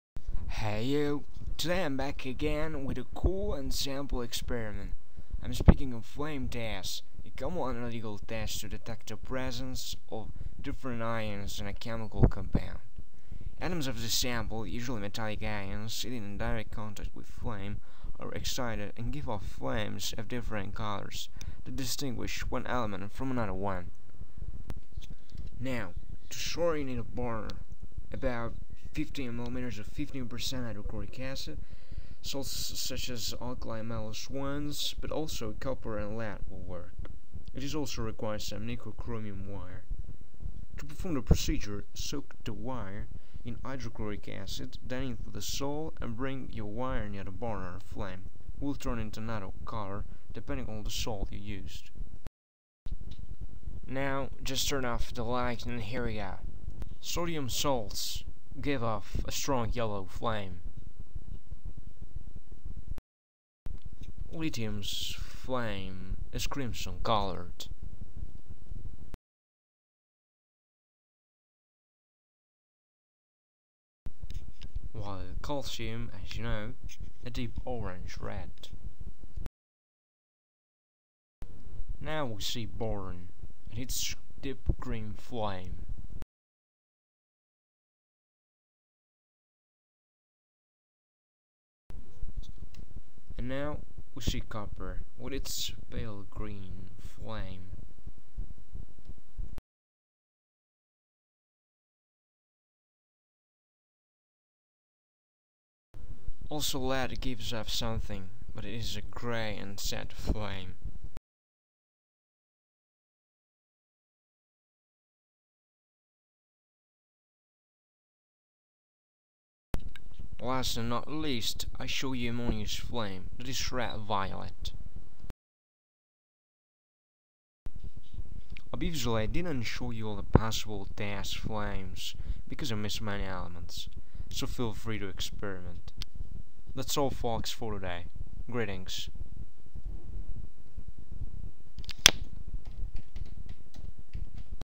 FLAME TESTS
Hey you, today I'm back again with a cool and simple experiment. I'm speaking about flame test, a common analitical test ti detect the presence of different ions in a chemical compound. Atoms of th sample(usually metallic ions) heated in direct contact with flame are excited and give off flames of different colours that distinguish one element from another one.
Now, to start you need a burner, about 50 ml of 15% HCl acid, salts such as alkali metals ones but also copper and lead will work. It is also required some nickel-chromium wire.
To perform the procedure soak the end of the wire in hydrochloric acid, then into the salt and bring your wire near the burner flame:it will turn into another colour depending on the salt you used.
To show you better the colour I will turn off the light. And here we go.
Na,Li,Ca,B,Cu,Pb,-NH4
Obiviously I didn't show you all tests flames because I miss many elements. So feel free to experiment.
That's all folks, greetings.